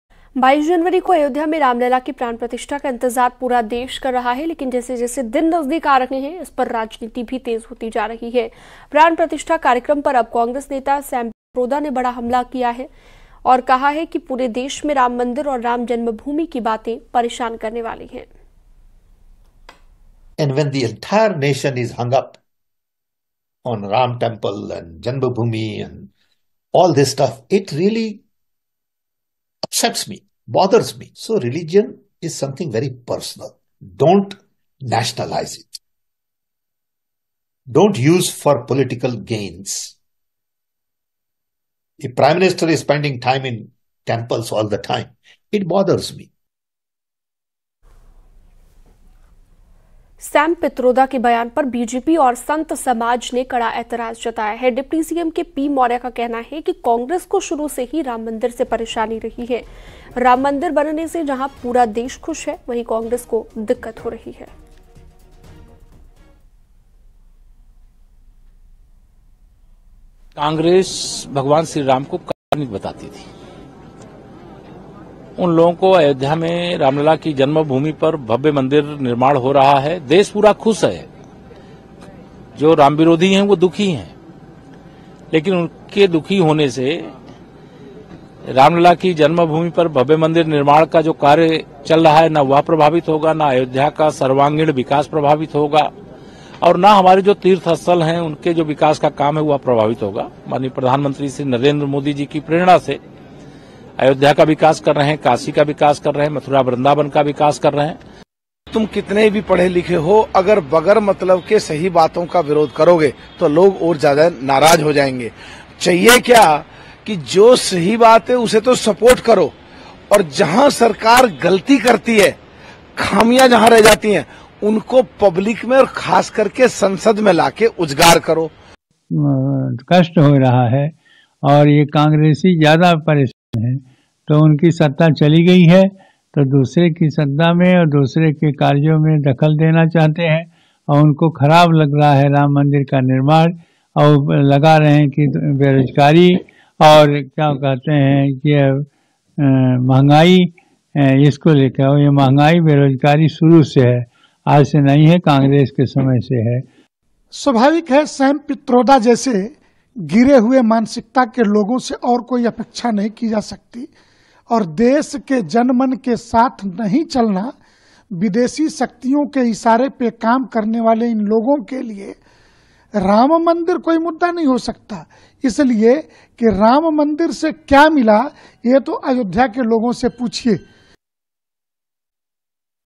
Ram Mandir Pran Pratishtha पर Sam Pitroda का विवादित बयान। Keshav Prasad Maurya | PM Modi
इंडियन ओवरसीज कांग्रेस के चेयरमैन सैम पित्रोदा ने कहा कि धर्म निजी विषय है, इसका राष्ट्रीयकरण मत करिए.राजनीतिक फायदे के लिए इसका इस्तेमाल मत करिए. ..